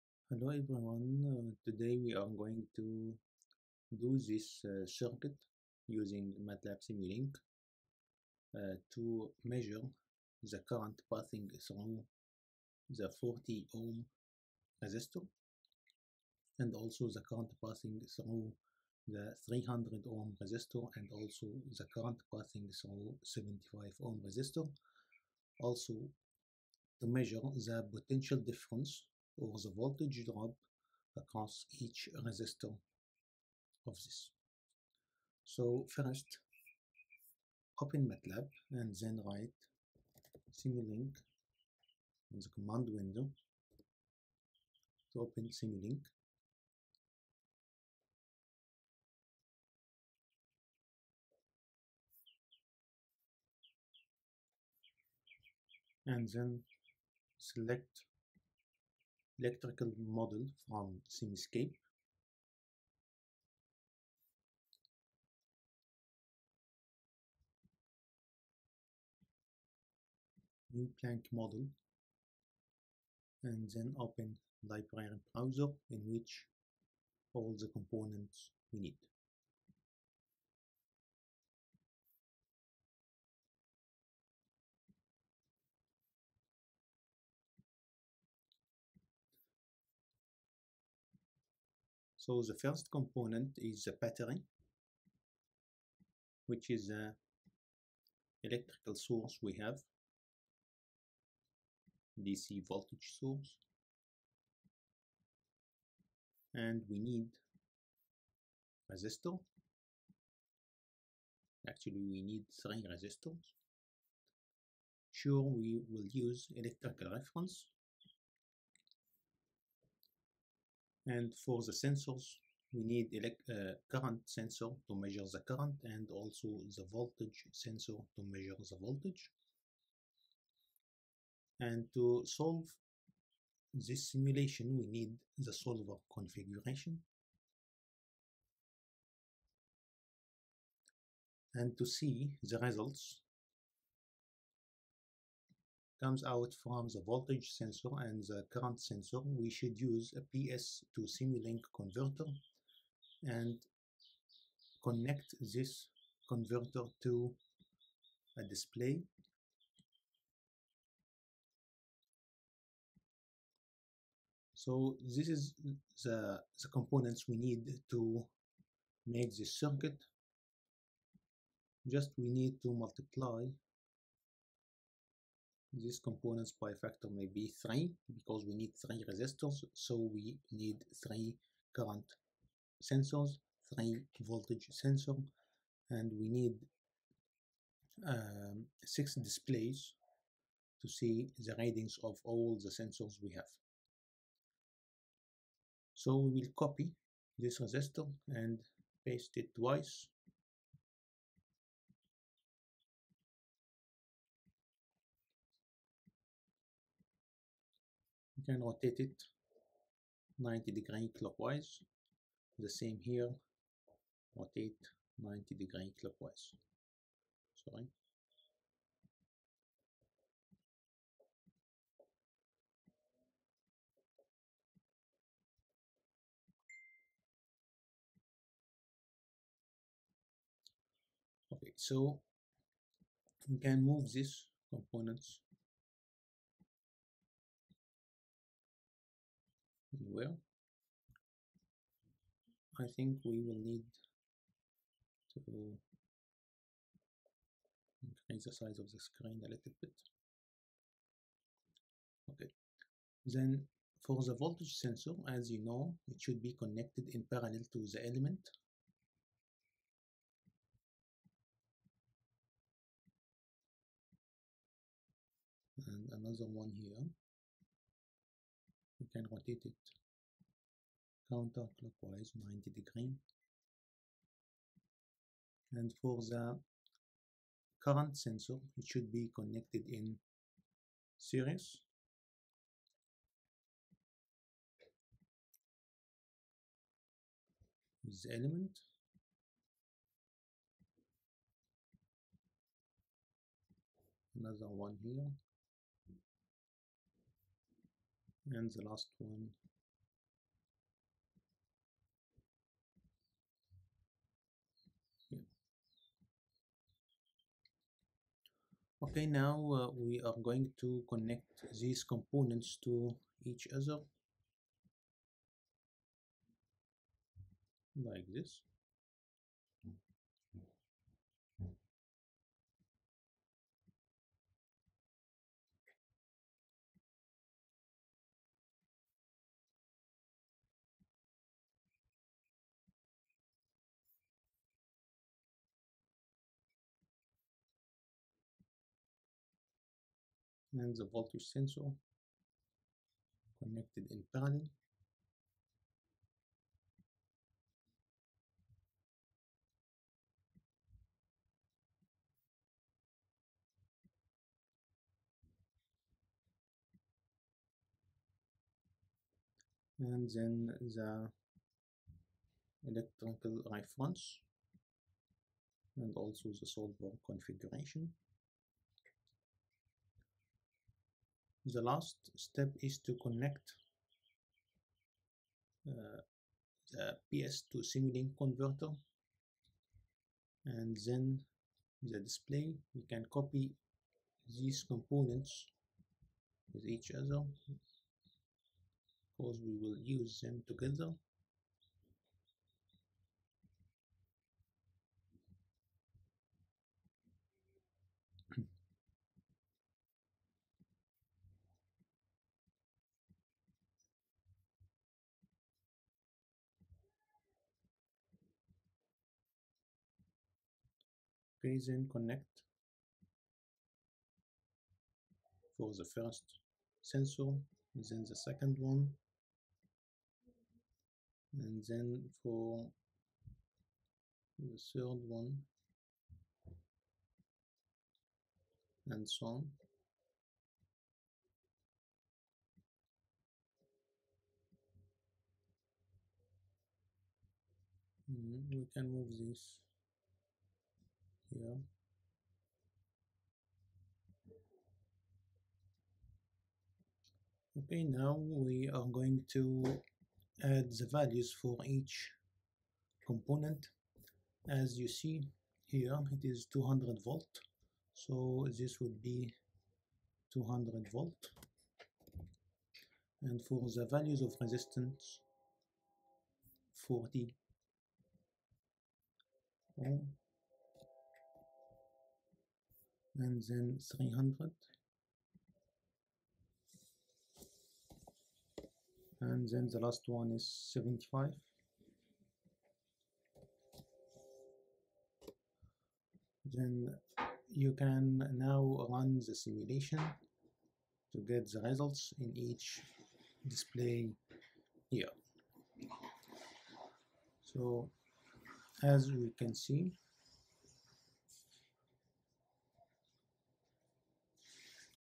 MATLAB Simulink Simscape Simple Electrical Circuit Simulation 2
By the end of this video, you will be able to use MATLAB Simulink to do Electrical Circuit Simulation.